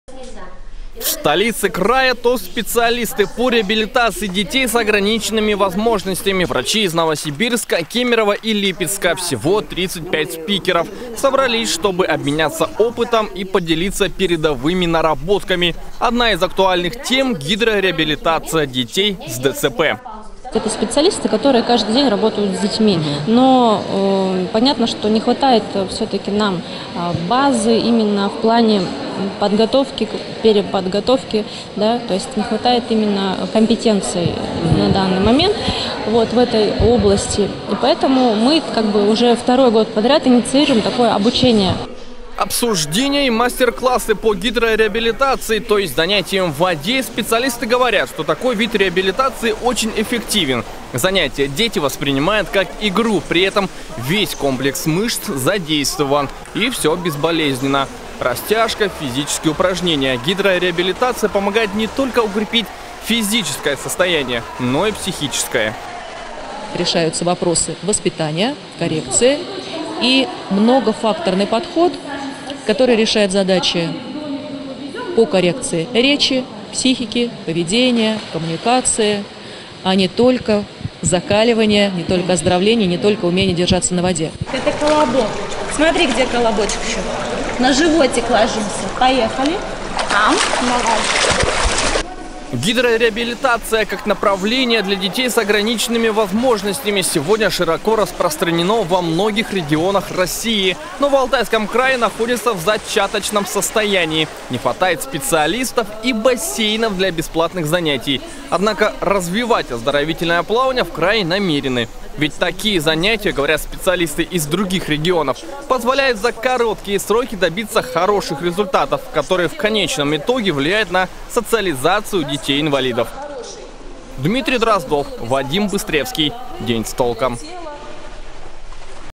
Помочь детям: в Барнауле со всей России собрались специалисты по оздоровительному плаванию
В Барнауле со всей России собрались специалисты по оздоровительному плаванию. Они помогают детям с ДЦП, аутизмом, синдромом Дауна и после травм. Если ли в Алтайском крае место гидрореабилитации? И каких результатов можно добиться с помощью этой методики?